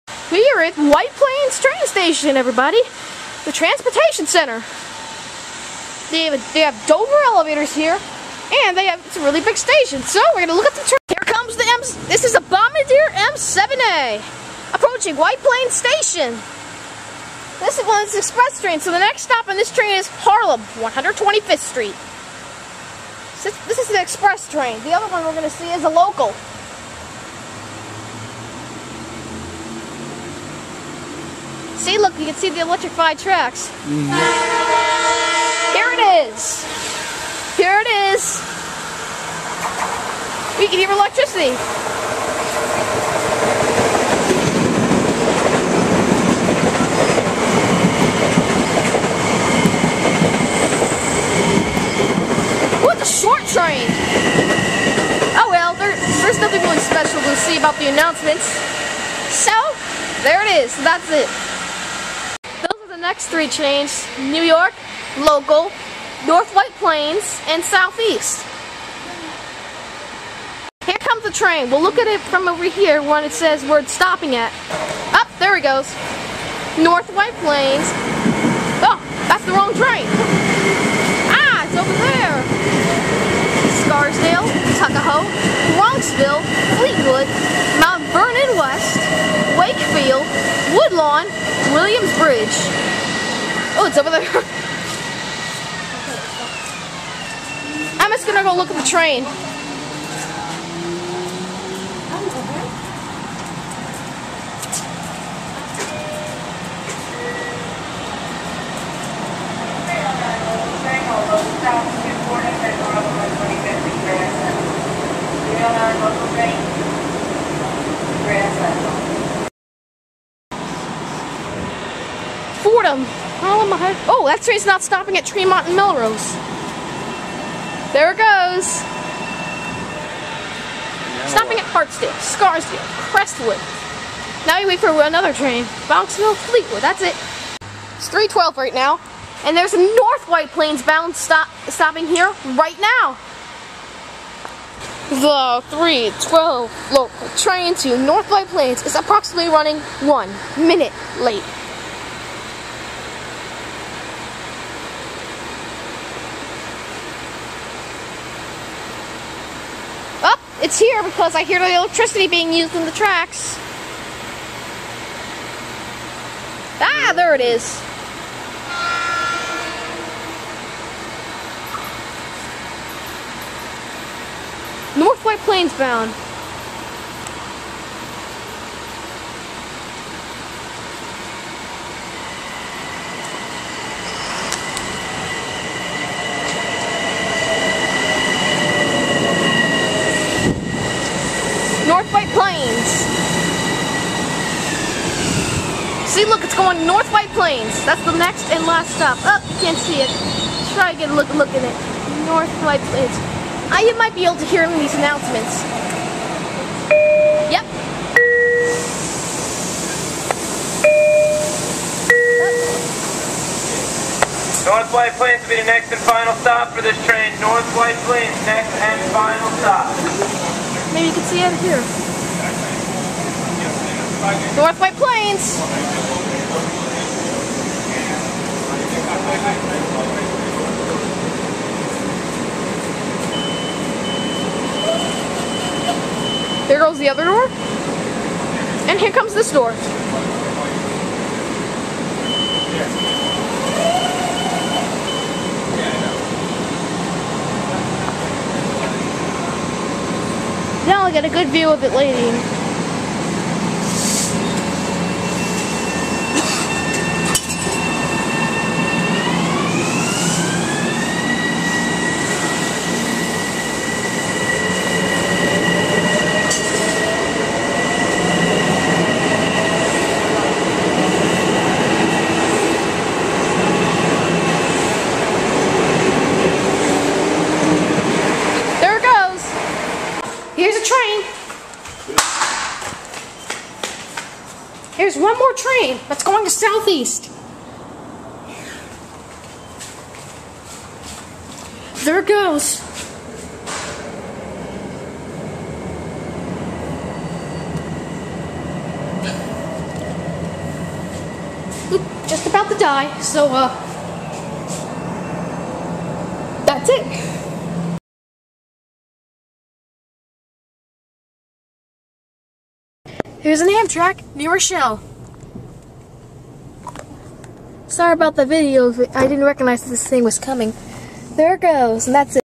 June 2012: 4 Bombardier M7A Trains At White Plains Harlem Line Metro North
This Is Another Backlog Railfanning Video